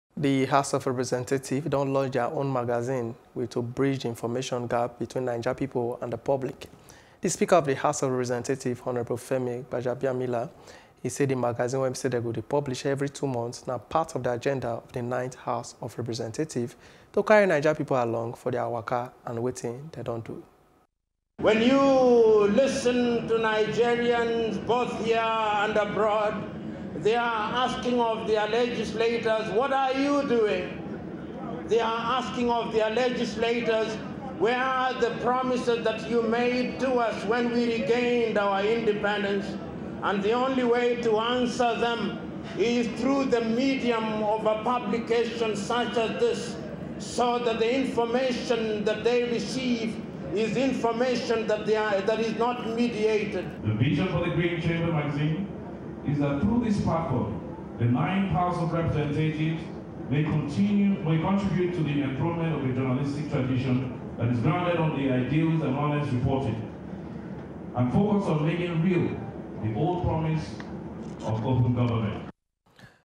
Information Sharing: House Of Reps Don Launch Own Magazine
The House of Representatives don launch dia own magazine with to bridge the information gap between Naija pepo and the public.